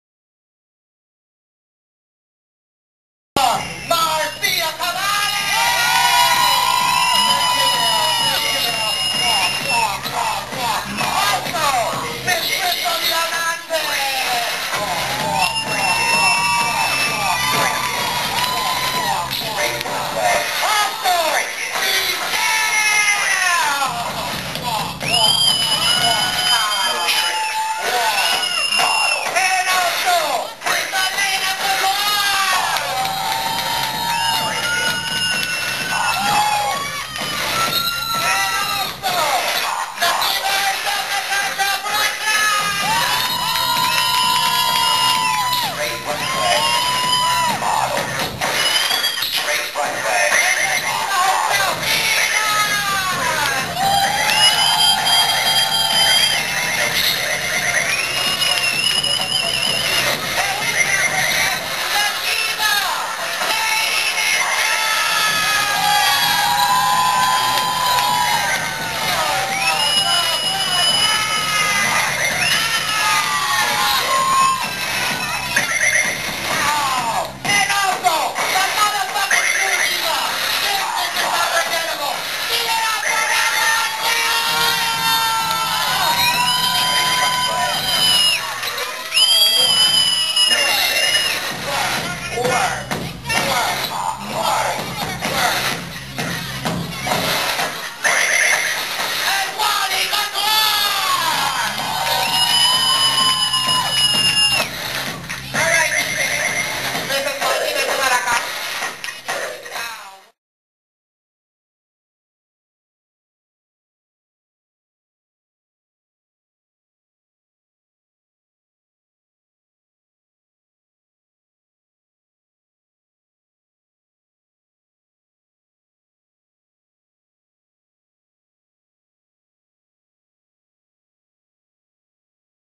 Sunday Madness Runway walk C-loz Bday bash
Sunday Madness Runway walk for  C-loz Bday bash feat. Sunday Madness Marcia Cavalli , Krystal Diamante , Giselle , Aleyna Couture , Natasha Brooklyn , Venus , Liyah Ninja , La Mas Mejor , Svadi Couture.